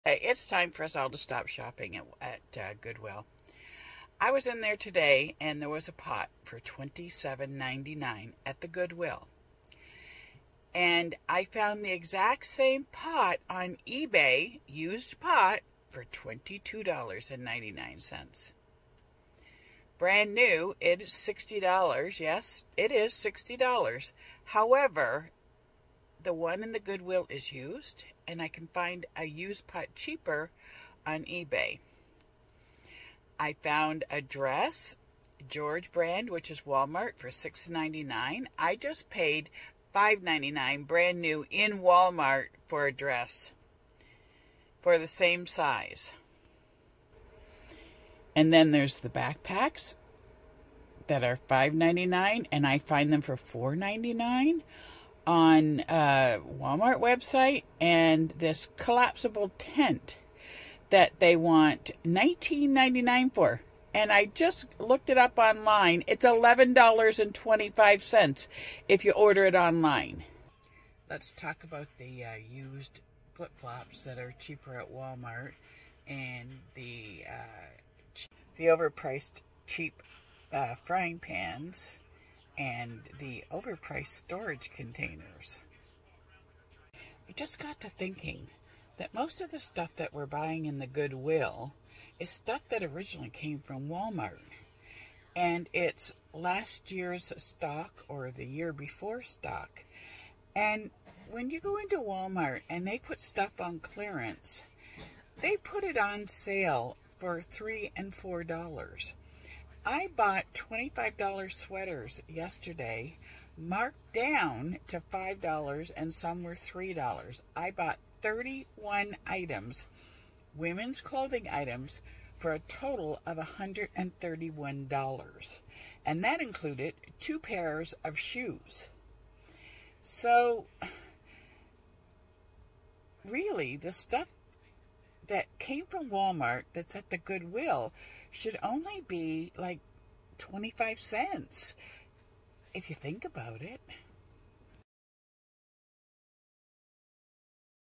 ARE GOODWILL PRICES TOO EXPENSIVE?
32 ITEMS PURCHASED AT WALMART ARE CHEAPER THAN GOODWILL. I will be thrifting elsewhere from now on.